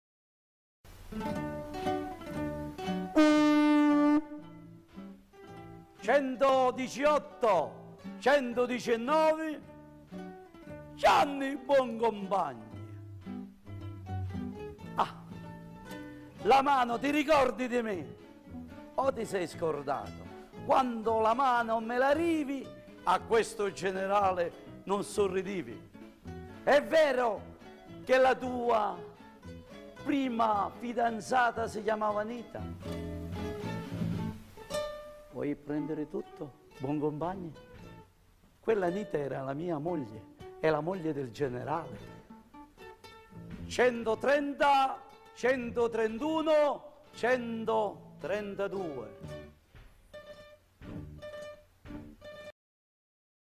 Garibaldi contro Gianni Boncompagni
Garibaldi non te le manda a dire. Durante la sua spedizione si ferma a contare i suoi 1000 Garibaldini. Tra i mille scova sempre qualche Garibaldino Vip. Stavolta è il turno di Gianni Boncompagni